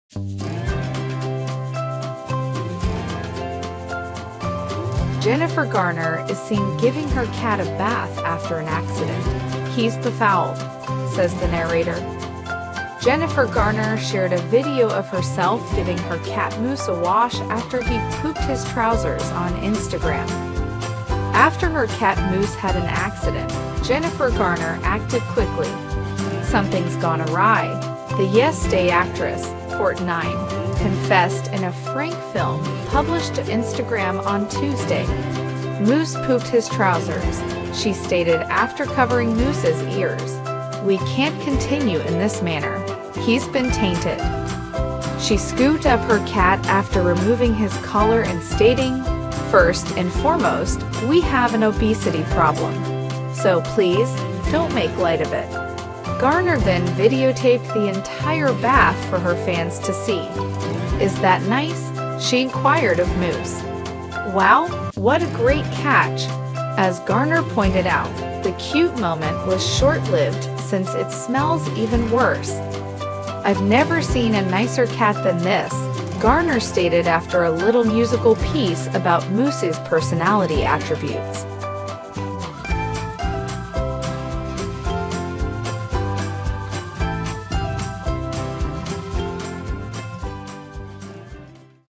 Jennifer Garner is seen giving her cat a bath after an accident: 'He's Befouled!' says the narrator.
Jennifer Garner shared a video of herself giving her cat Moose a wash after he "pooped his trousers" on Instagram.

After her cat Moose had an accident, Jennifer Garner acted quickly. "Something's gone awry," the Yes Day actress, 49, confessed in a frank film published to Instagram on Tuesday. "Moose pooped his trousers," she stated after covering Moose's ears. We can't continue in this manner. He's been tainted.